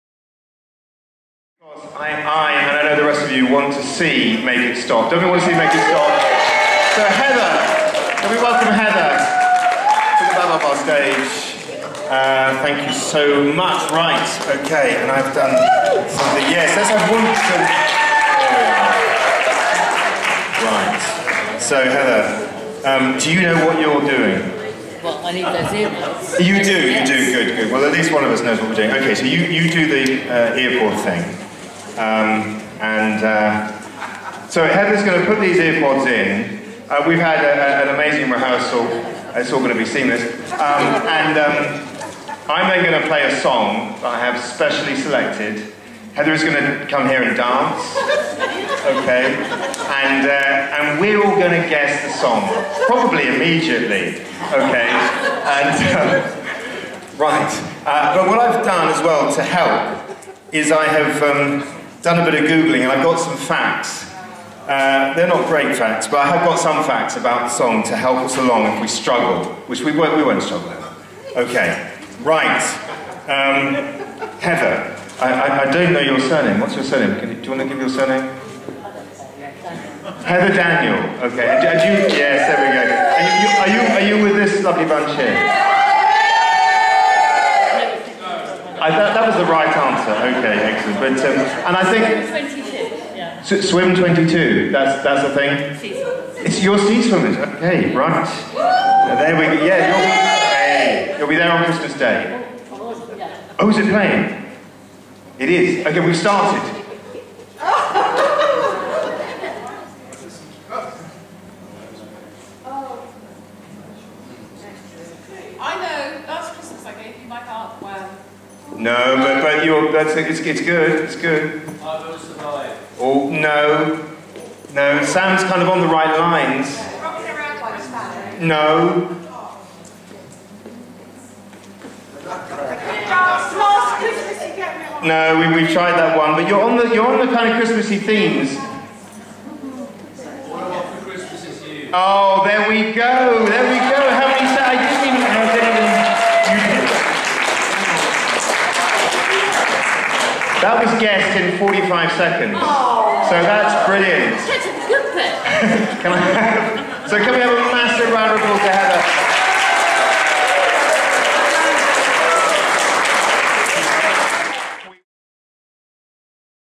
Make it Stop - Heather Daniel
Heather dances to a song only she can hear, while the audience have to guess what it is...